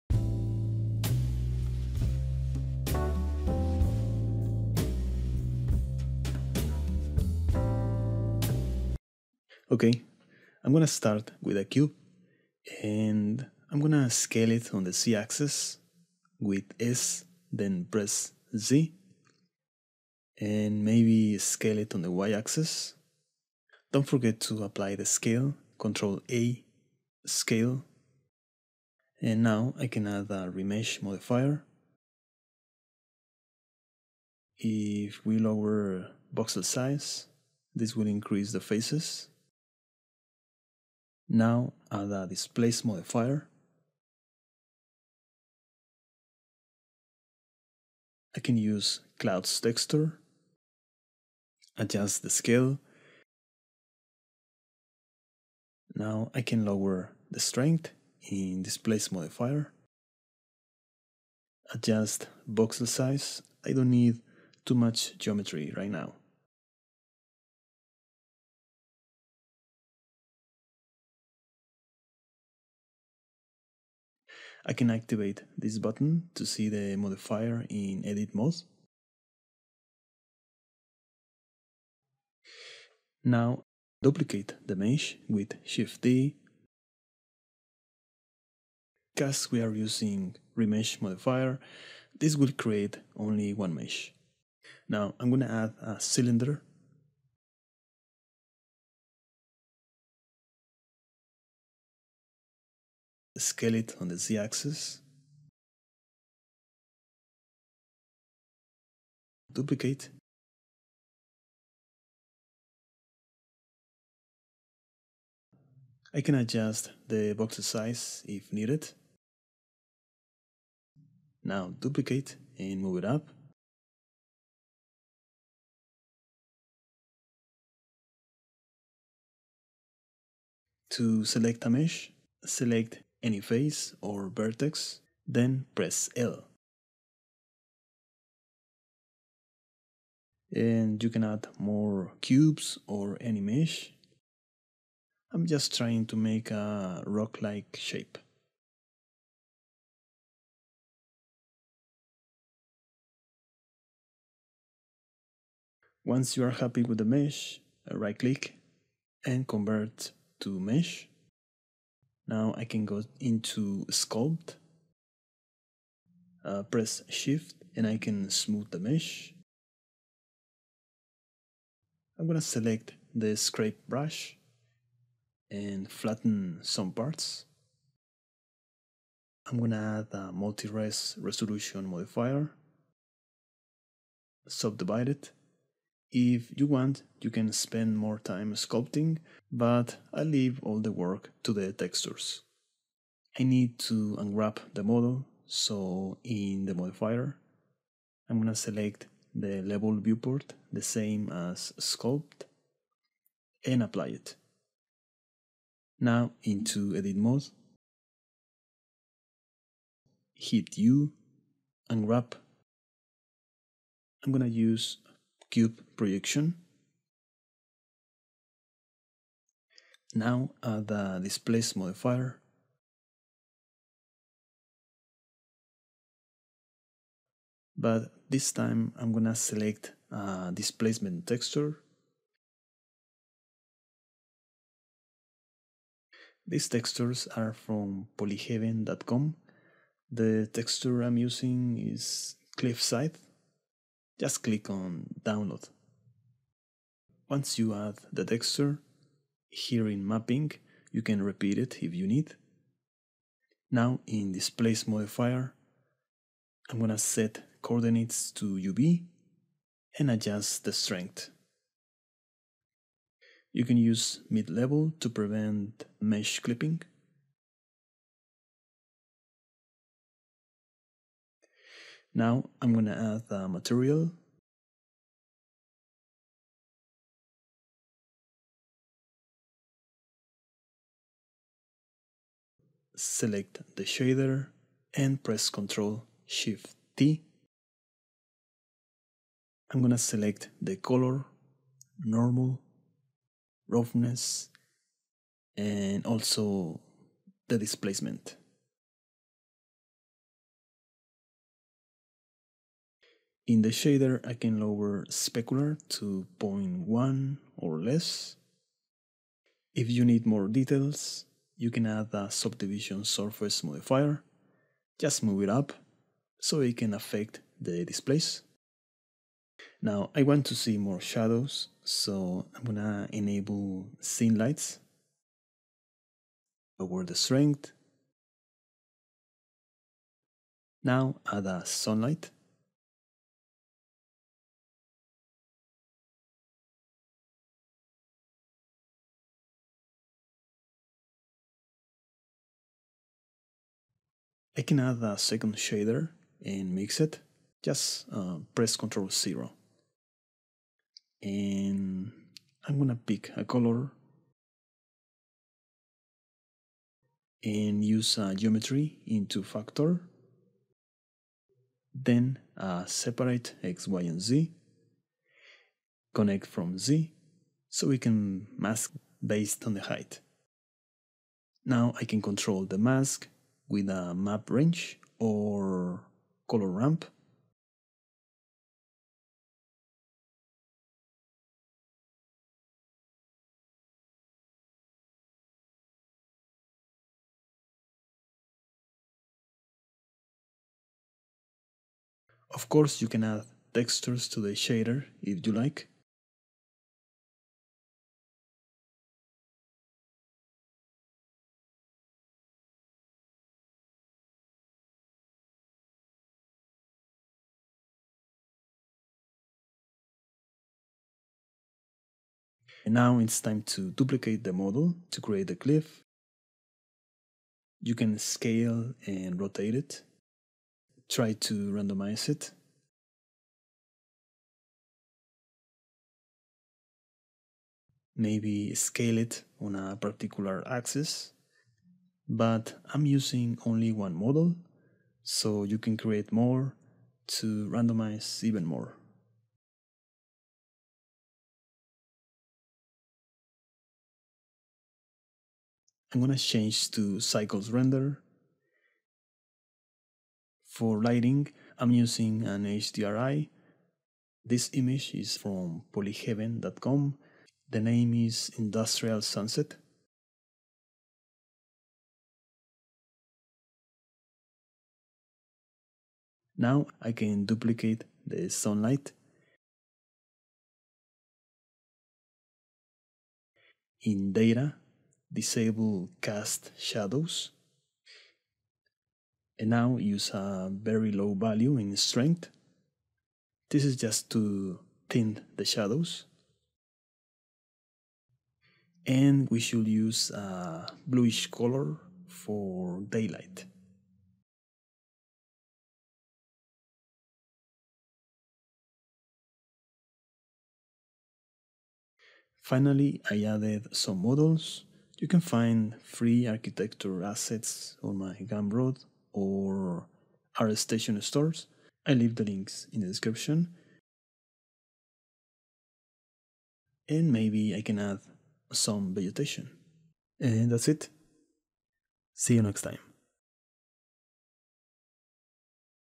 How To Create Realistic Cliffs and Rocks in Blender
In this video I will show you how to create realistic Rocky Mountains, Cliffs, Canyons for your Landscapes in Blender

Textures and HDR from polyhaven.